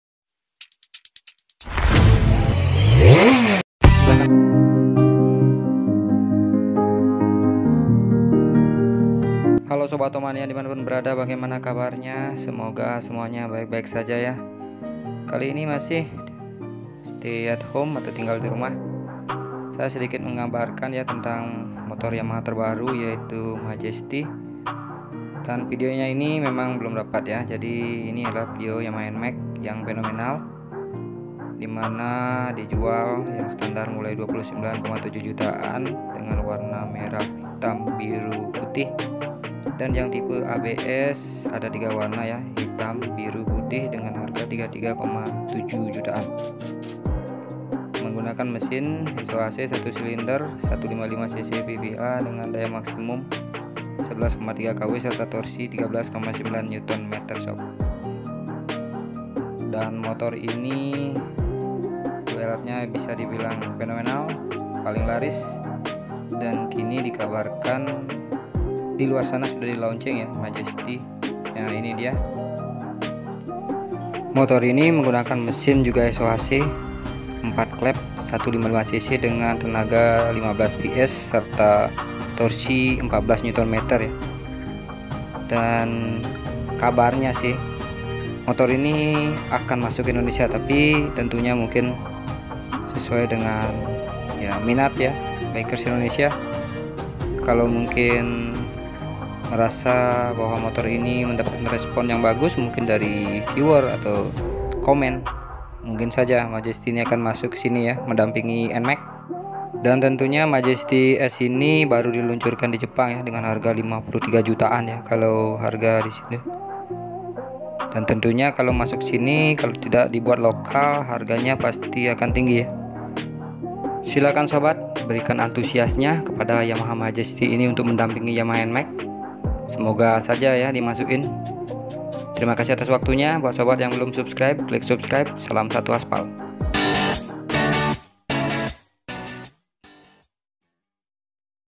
Yamaha Majesty siap dampingi New Yamaha Nmax
Yamaha Nmax dikabarkan akan mendapat saudara baru yaitu yamaha majesty. Namun untuk diindonesia masih dalam perencanaan. Mungkin jika antusias bikers besar,pastilah yamaha majesty bakal masuk.